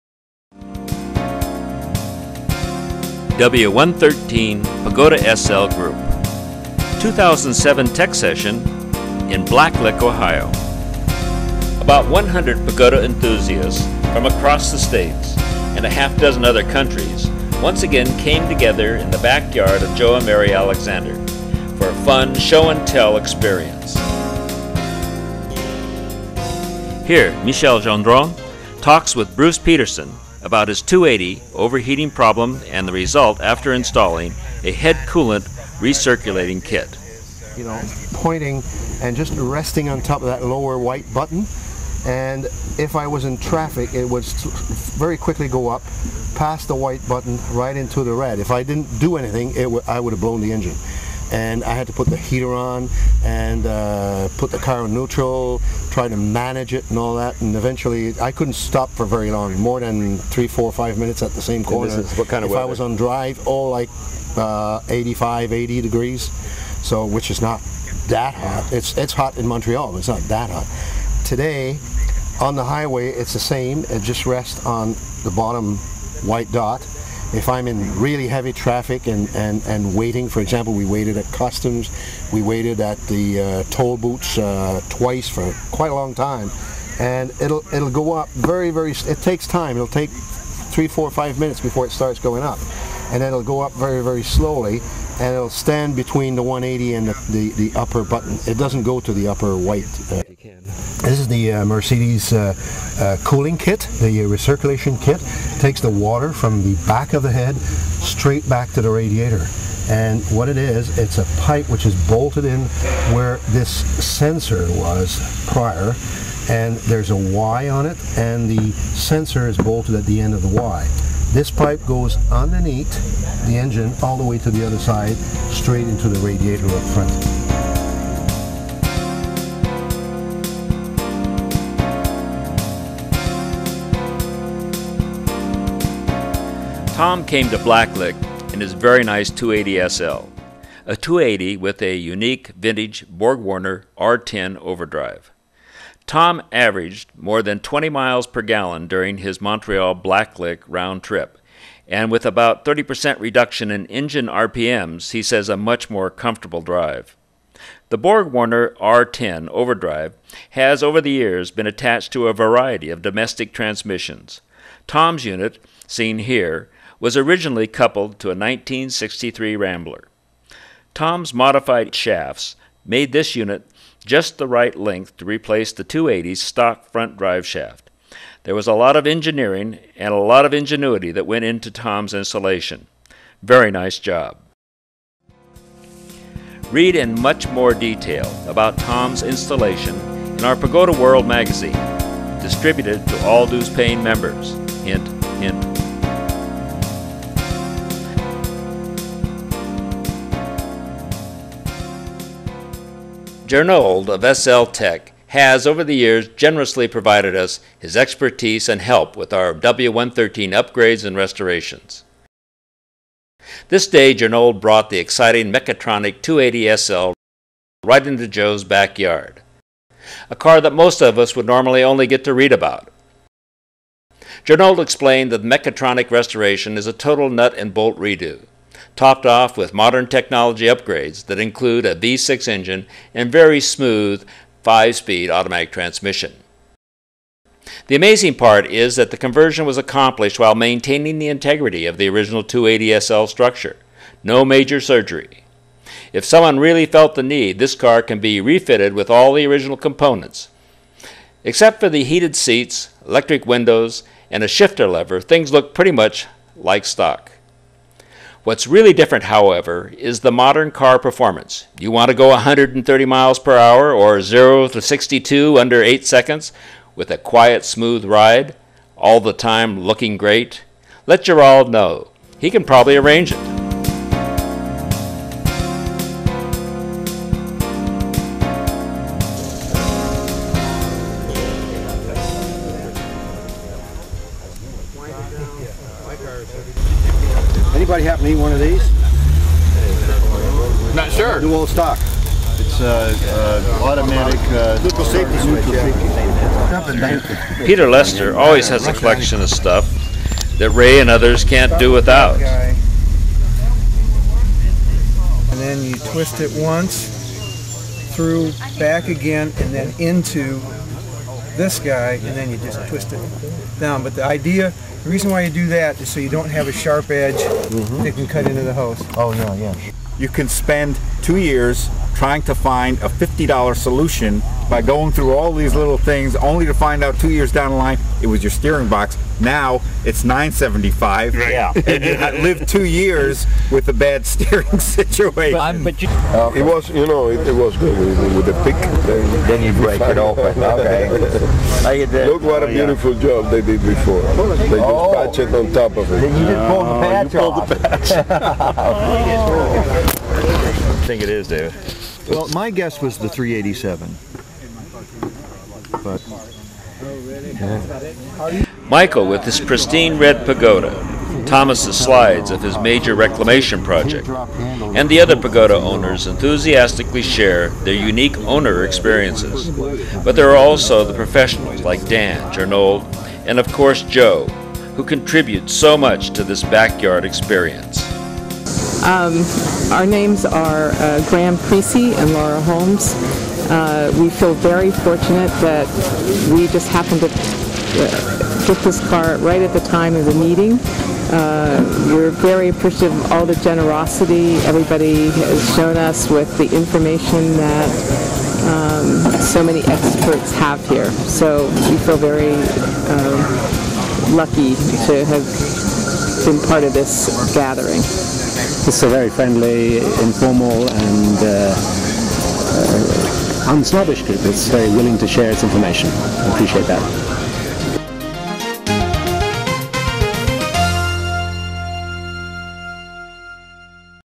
W113 Pagoda Blacklick Show & Tell
W113 Pagoda SL Group 2007 Tech Session Blacklick OH. Participants exchange information about their cars.

Dick Mentzer
Producer / Editor: Dick Mentzer
Videographer: Ray Hays